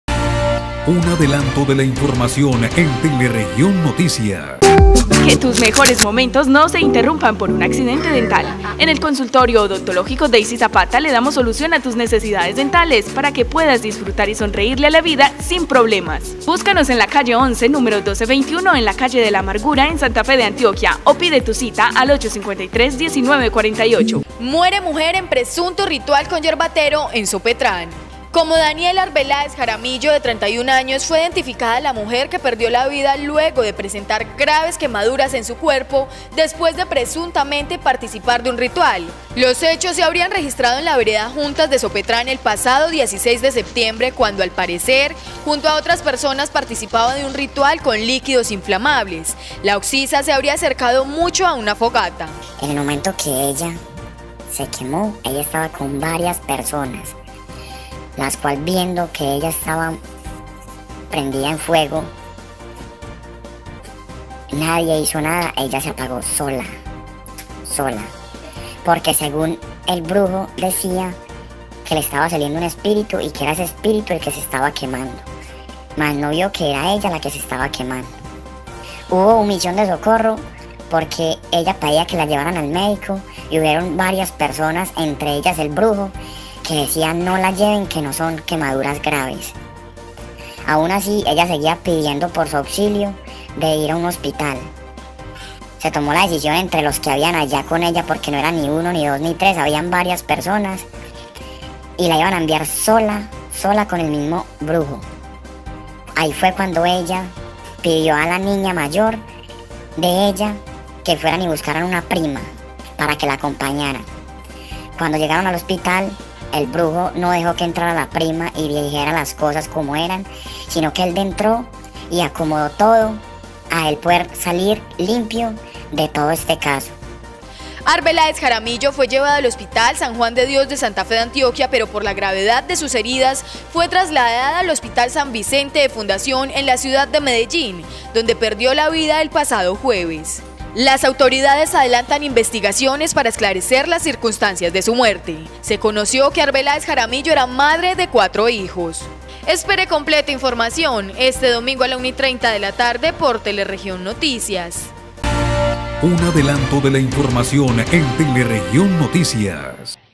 Muere mujer de Sopetrán en presunto ritual con yerbatero.
ADELANTODELAINFORMACIÓN – Muere mujer de Sopetrán en presunto ritual con yerbatero.